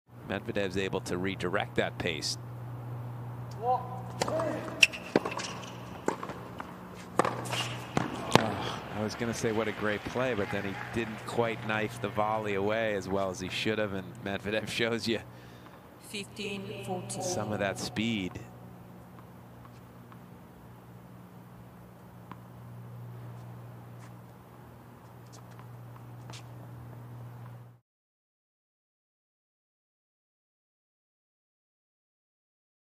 Daniil Medvedev's stunning crosscourt forehand winner! | US Open 2020 Hot Shots
Daniil Medvedev produces a stunning crosscourt forehand winner against Frances Tiafoe in Round 4 of the US Open 2020!